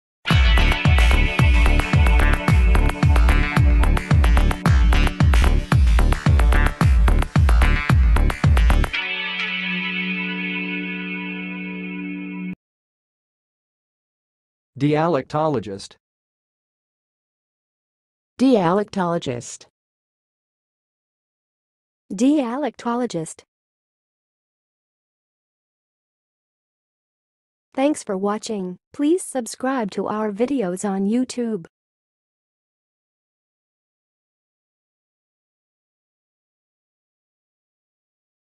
How to say "dialectologist"! (High Quality Voices)
Watch in this video how to say and pronounce "dialectologist"!